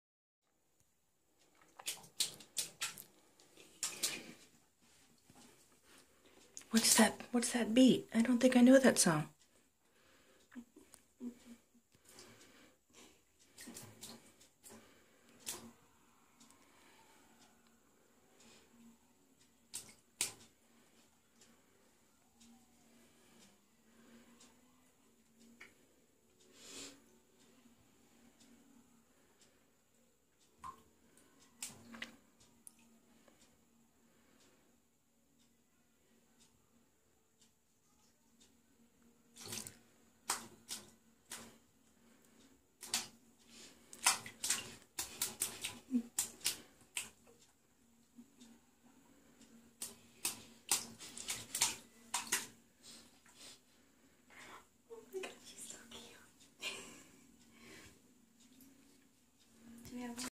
Bunny Grooving to the Beat
Lylah the runny babbit grooving to the beat of the water drums during bath time